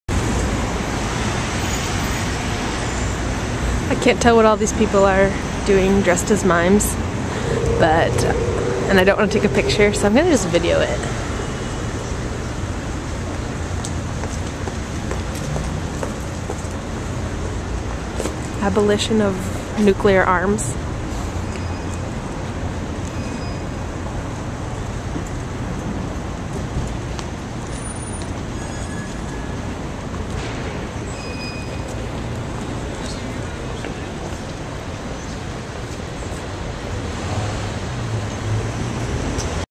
SILENT PROTEST (PARIS)
VIDEO JOURNAL EPISODE 169: Yet another protest in Paris... but this one was silent, with masks.

TRAVEL ITINERARY: from Phoenix Arizona (USA) to Colombia, Ecuador, Peru, Bolivia, Chile, Brazil, Ireland, and England for 6 months... from Sept 2013 - March 2014.